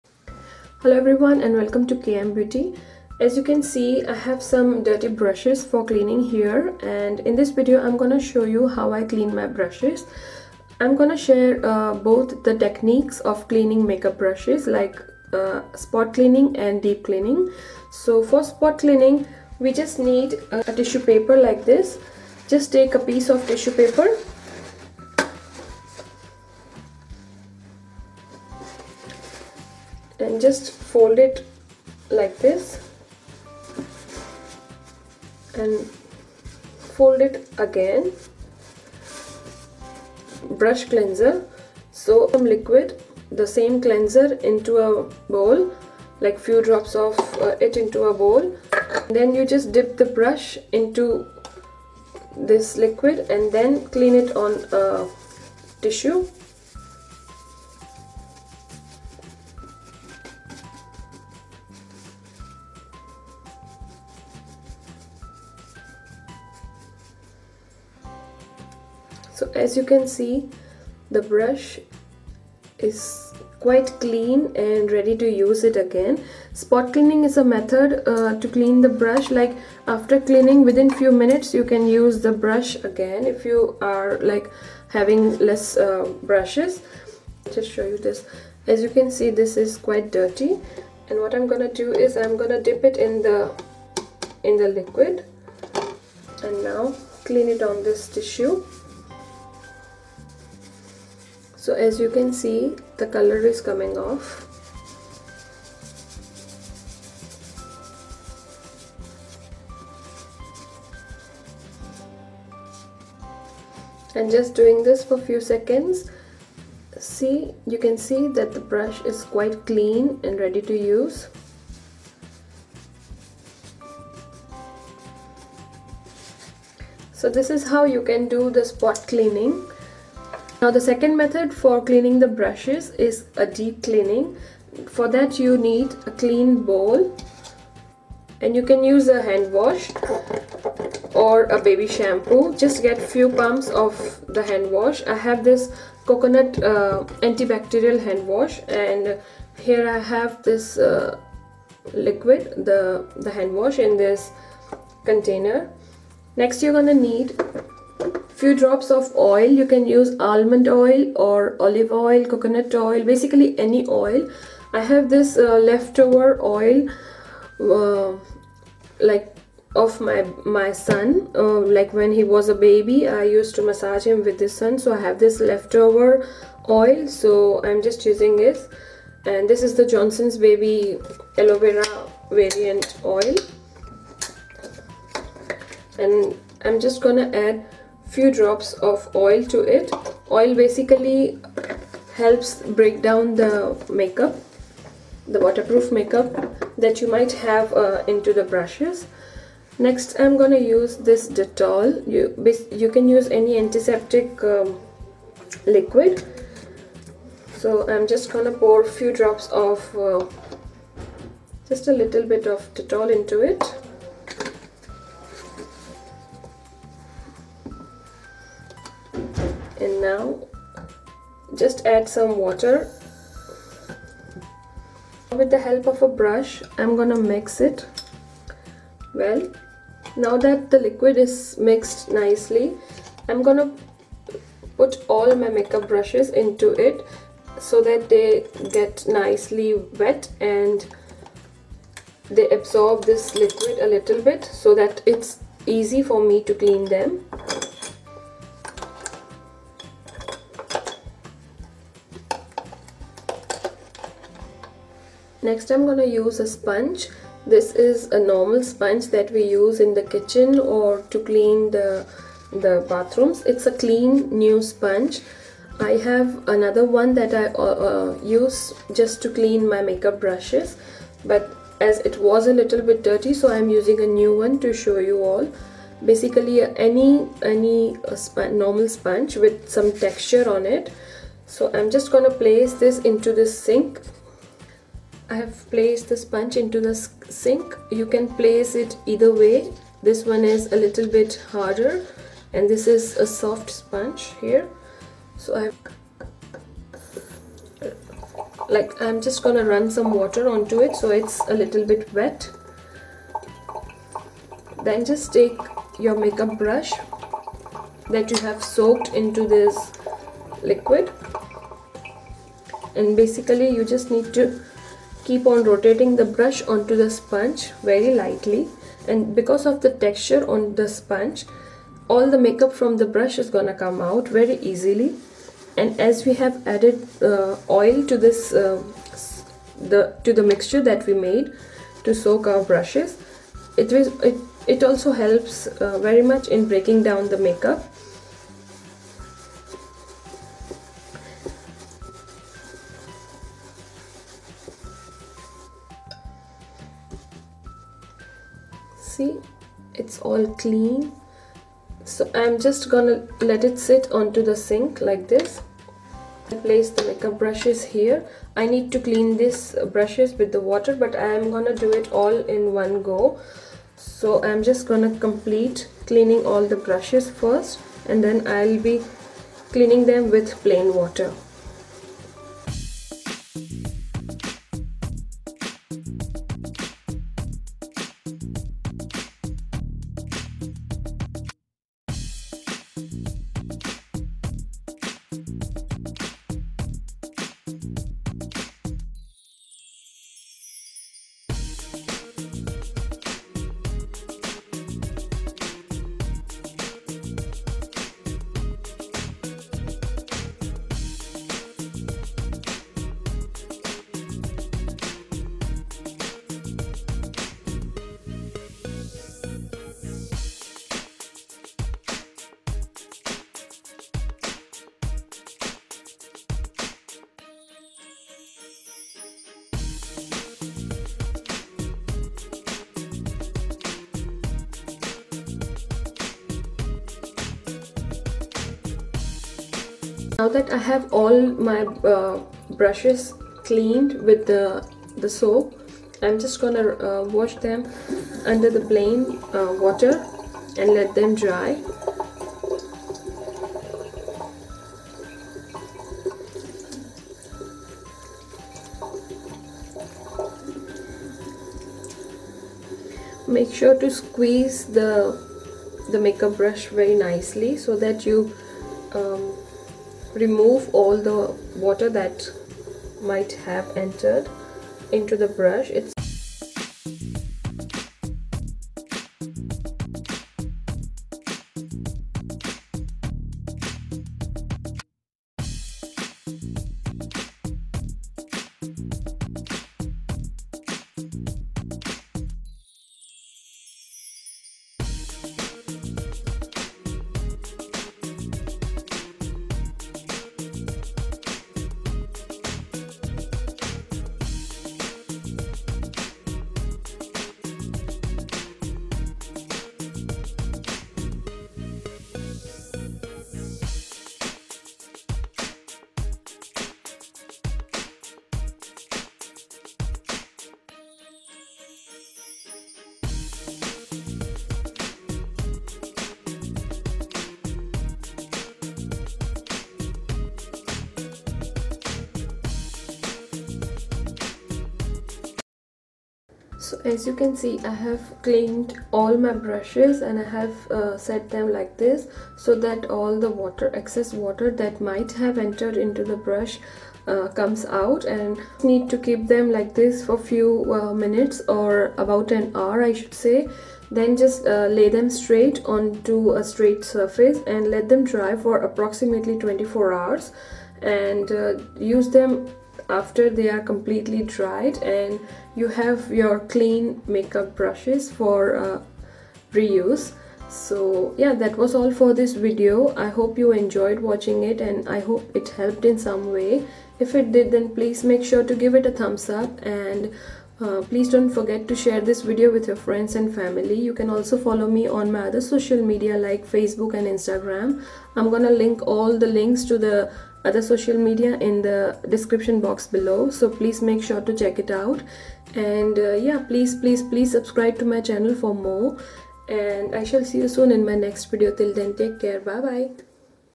How to clean your Makeup Brushes / Cheapest and Easiest Way!
Hello Everyone, here m showing u all how i clean my makeup brushes, i've been using this method for a very long time now and so far i've found that this is the most cheapest and easiest way to clean my makeup brushes..hope u all will like it and hope that it helped u in some way, please do LIKE & SHARE this video.

Thank u